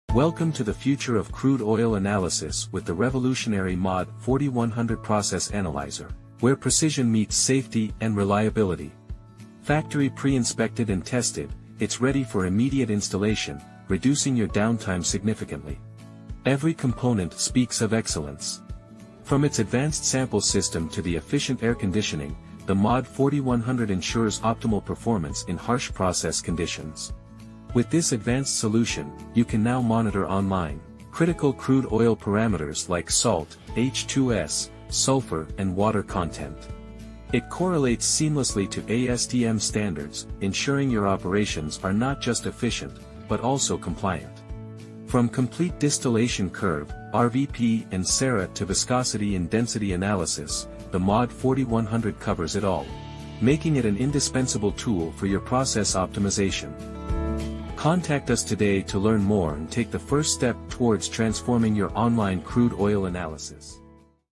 MOD-4100 Crude Analyzer 3D
Crafted with the cutting-edge "Modular Package concept," MOD-4100 crude analyzer offers unparalleled versatility, capable of performing a wide range of crucial crude oil measurements. It delivers critical, real-time analytical data that is vital for optimizing operational processing in the oil and gas industry. The MOD-4100 is more than just a process analyzer; it's a game-changing tool designed to enhance efficiency and sustainability in petroleum industries.
Discover more about how the MOD-4100 can transform your operations